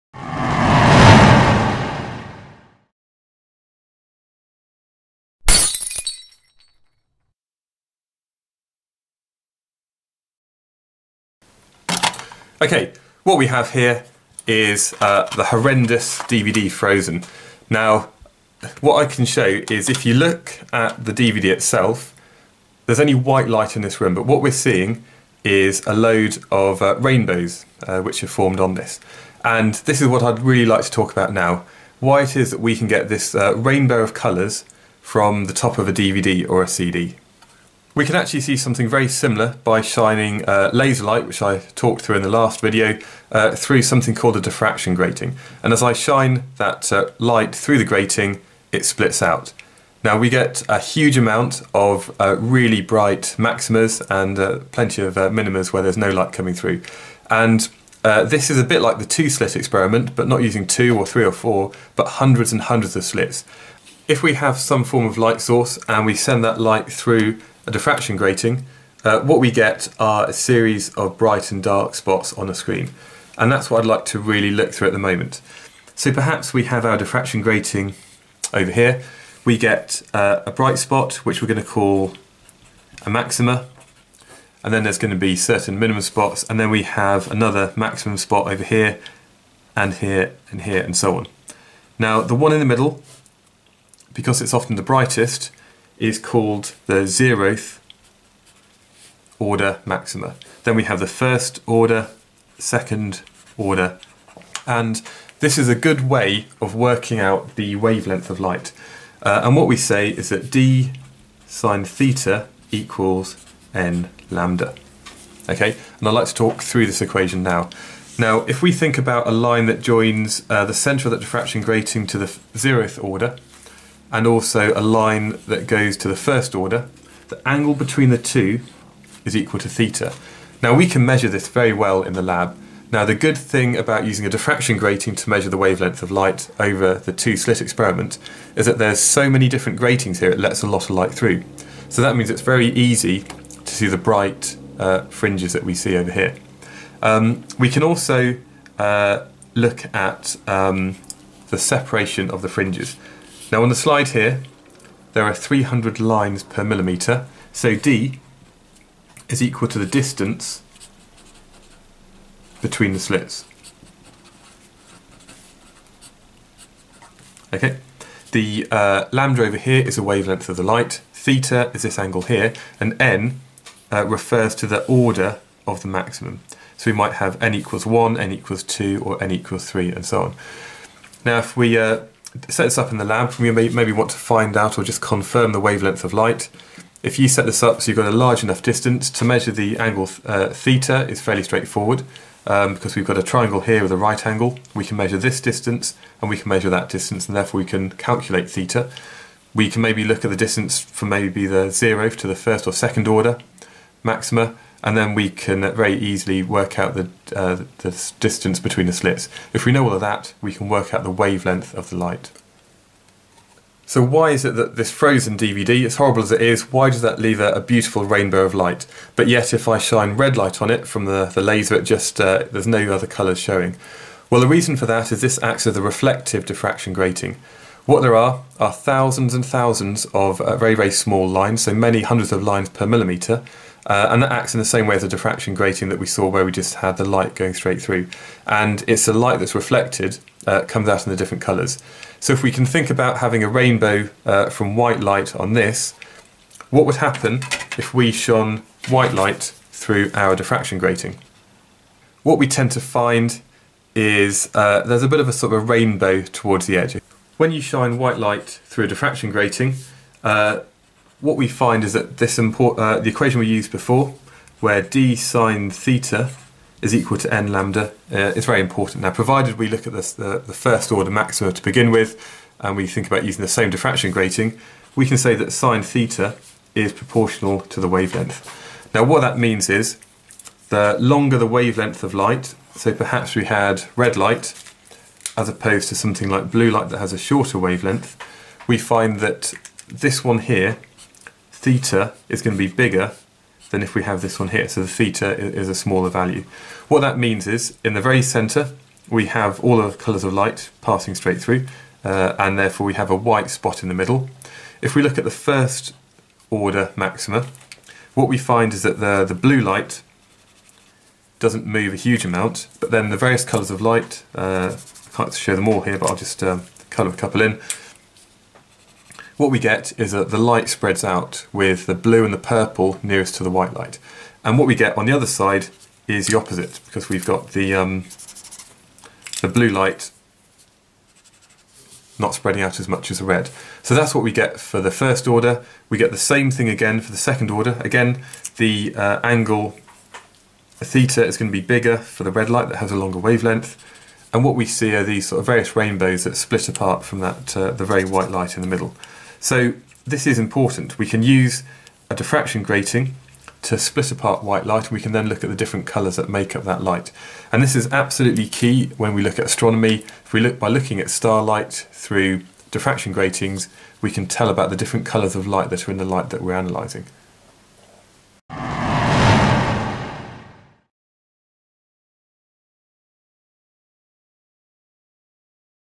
Diffraction Gratings - A Level Physics
This video introduces and explains diffraction gratings for A Level Physics.

If you shine light, or other types of EM radiation, through a series of small slits, the waves interfere causing constructive and destructive interference at certain points. We can then use this method to calculate the wavelength of light. It is also why DVDs and CDs make beautiful rainbows.

Lewis

AQA
CIE
Edexcel
Edexcel IAL
Eduqas
IB
OCR A
OCR B
WJEC